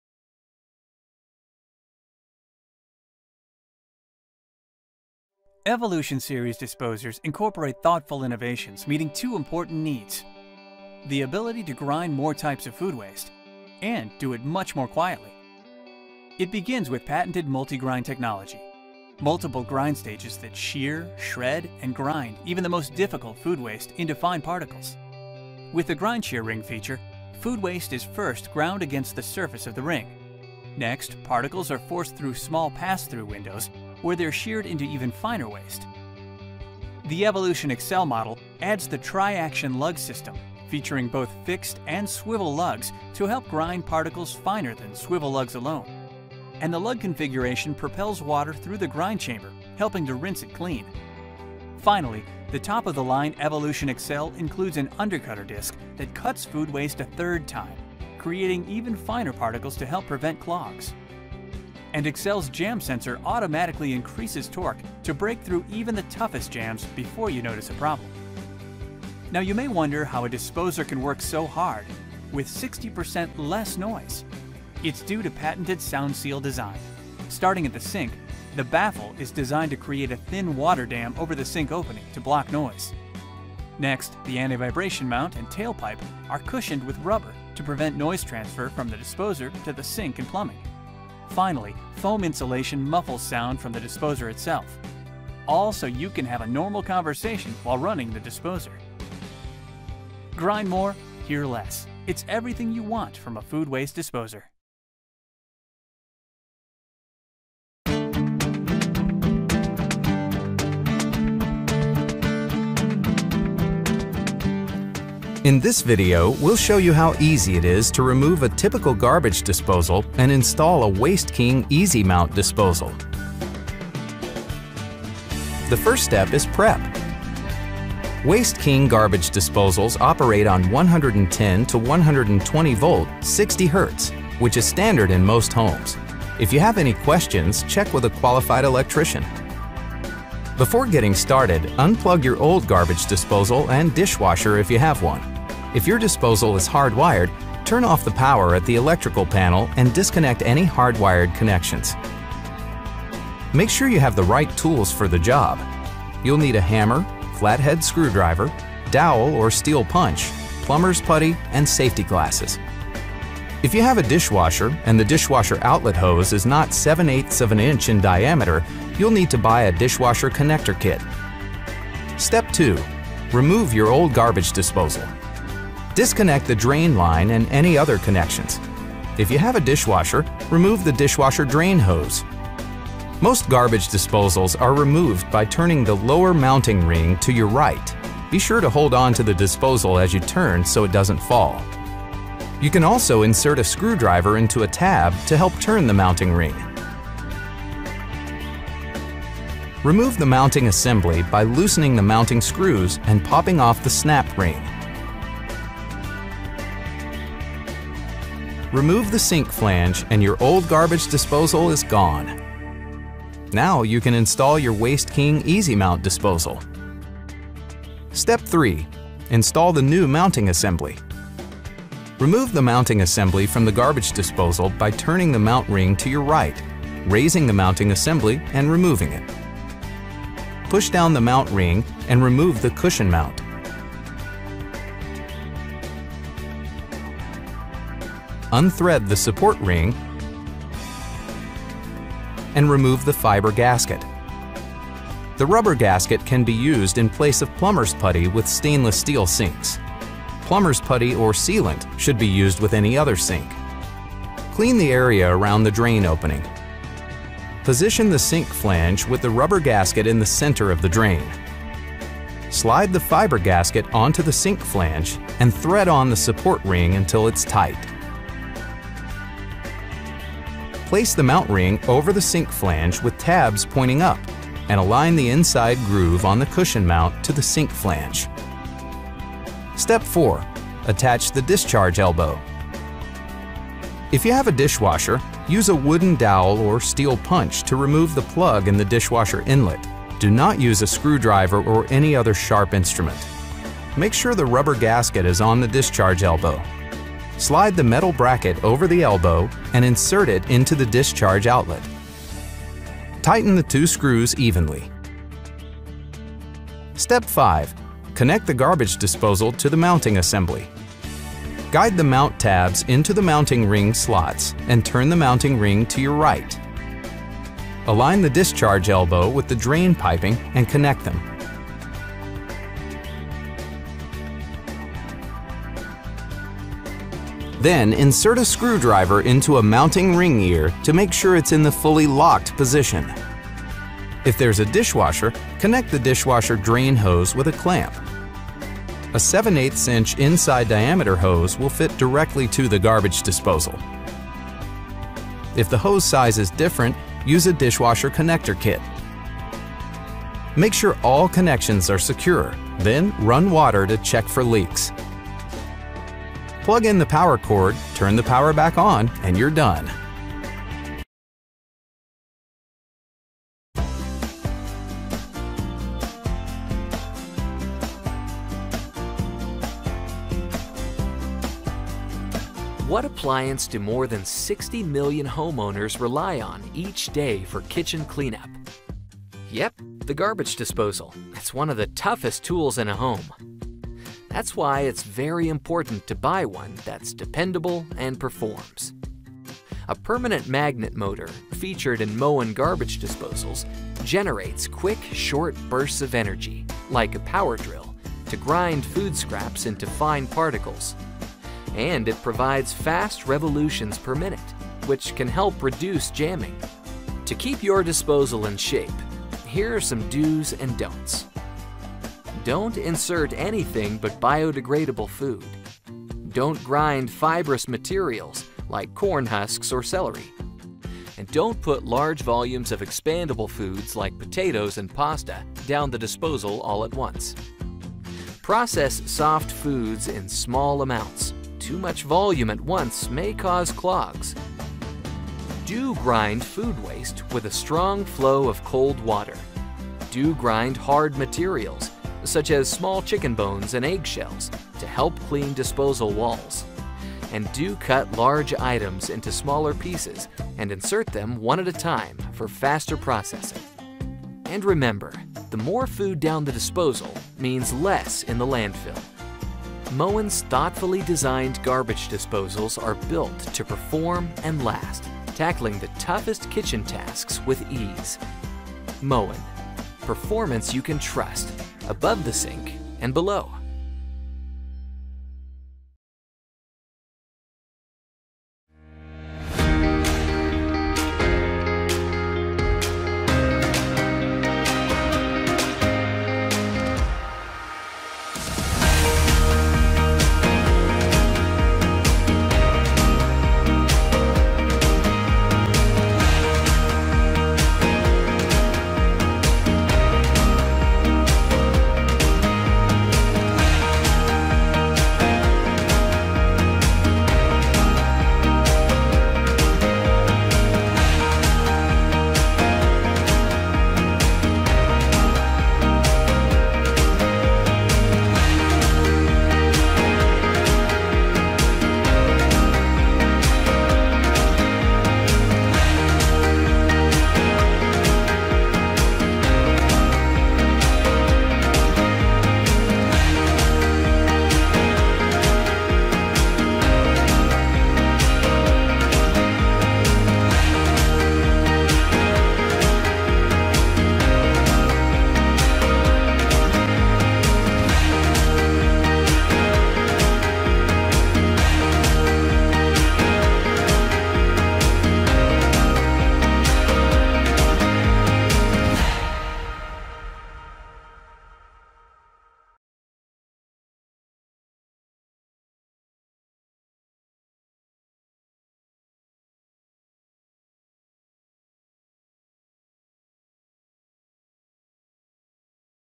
The Best Garbage Disposals of 2023
The Best Garbage Disposals listed in this video:

✔ 1. InSinkErator Garbage Disposal

✔ 2. Waste King L-111 Garbage Disposal

✔ 3. Moen GXS75C Garbage Disposal

✔ 4. InSinkErator Evolution Compact Garbage Disposer

Utilizing a garbage removal is harmless to the ecosystem and keeps your kitchen clean. We led long stretches of exploration to track down the best garbage disposals, assessing each model on its pull, commotion, chamber material, and establishment process. Our best generally pick is the InSinkErator Badger 5, ½ HP Continuous Feed Garbage Disposal, since it offers ½ drive, has stirred steel parts, and is reasonable.

Here are the best garbage disposals to keep your pipes clear and your sink clean.